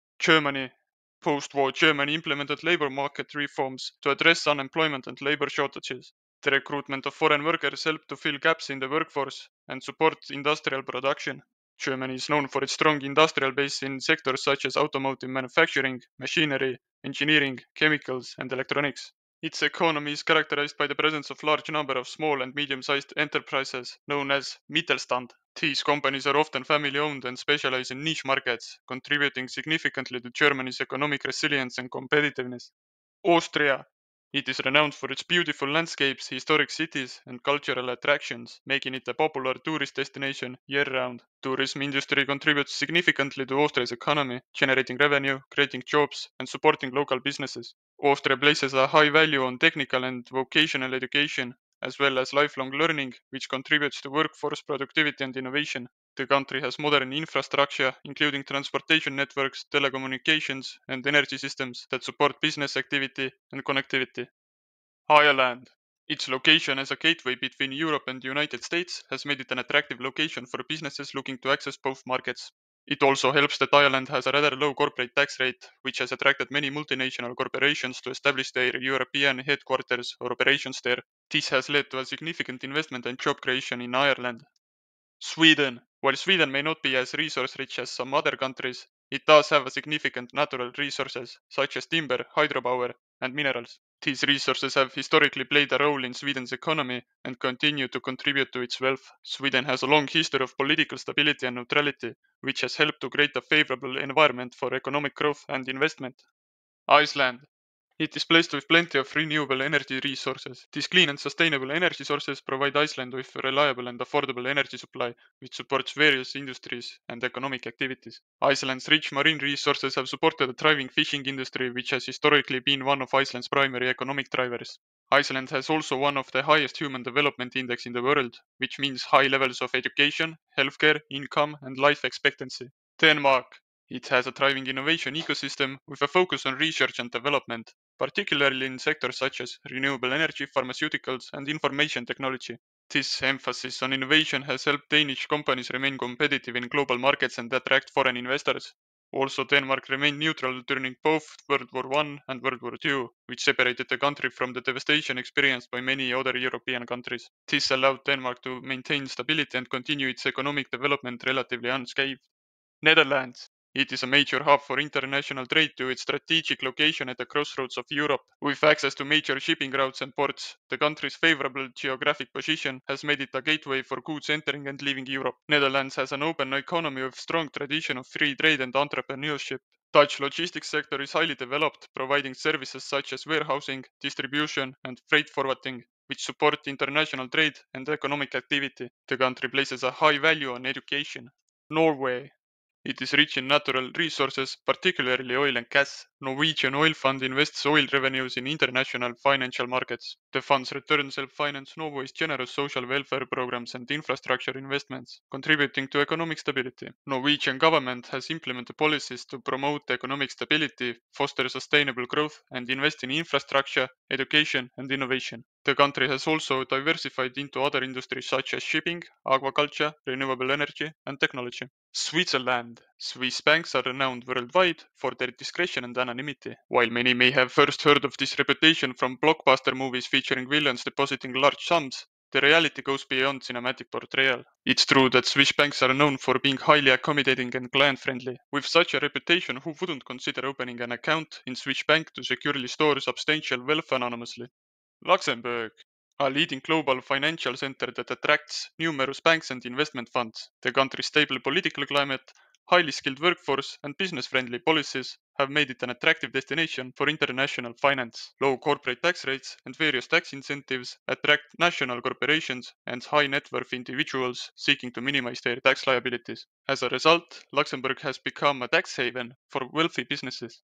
Richest European countries EXPLAINED in 5 minutes
👉10 wealthiest EU countries explained with simple words.👈